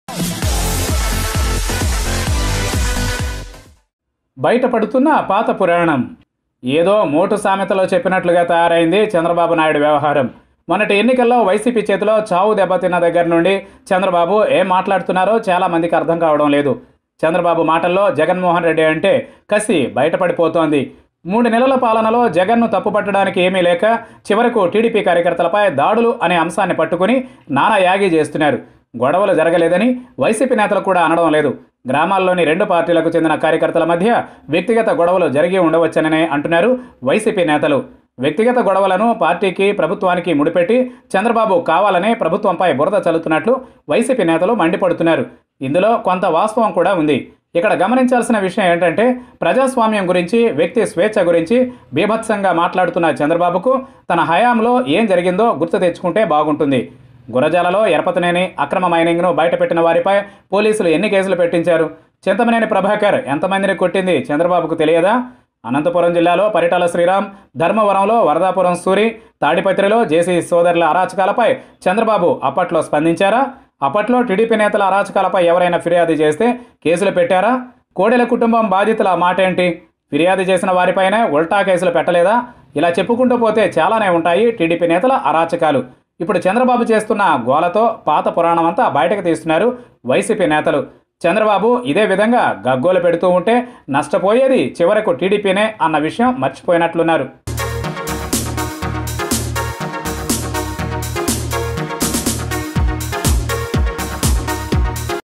ఛలో ఆత్మకూరుతో బయటపడుతున్నటీడీపీ పాత పురాణం| New discussion in AP politics with TDP Chalo Atmakuru
Watch this video to know more details.